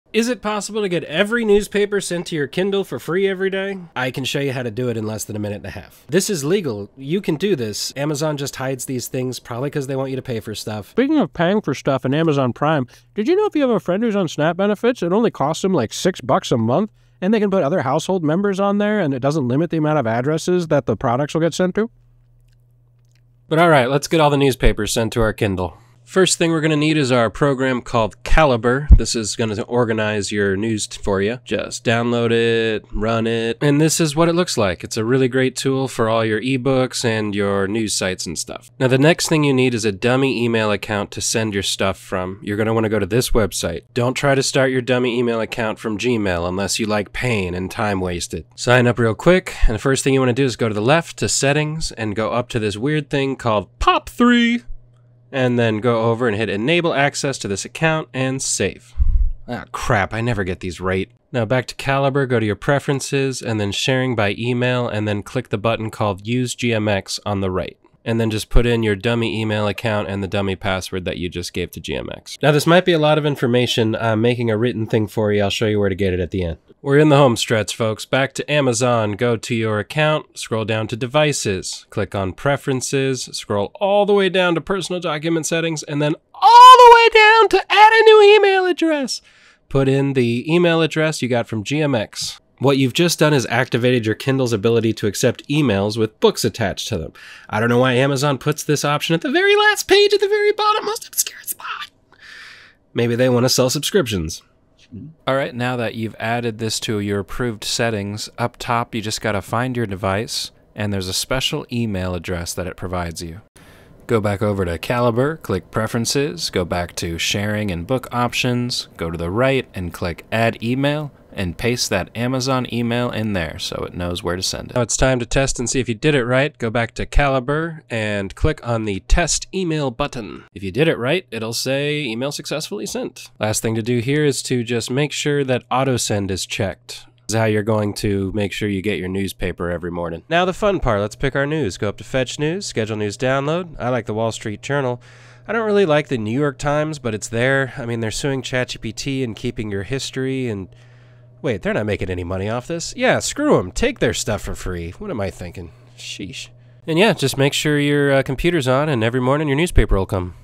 How to Get Free News on Your Kindle (No Subscription Needed)
Amazon doesn’t exactly advertise this, but your Kindle can deliver FREE daily news — no subscriptions required.

In this video, I’ll show you how to use Calibre’s “Fetch News” feature to:
• Set up automatic delivery of news feeds to your Kindle
• Organize and manage your favorite publications
• Wake up every morning with fresh news waiting for you

⚠️ This video uses Calibre’s built-in legal “Fetch News” function, which pulls from publicly available feeds. No piracy, no paywall bypassing.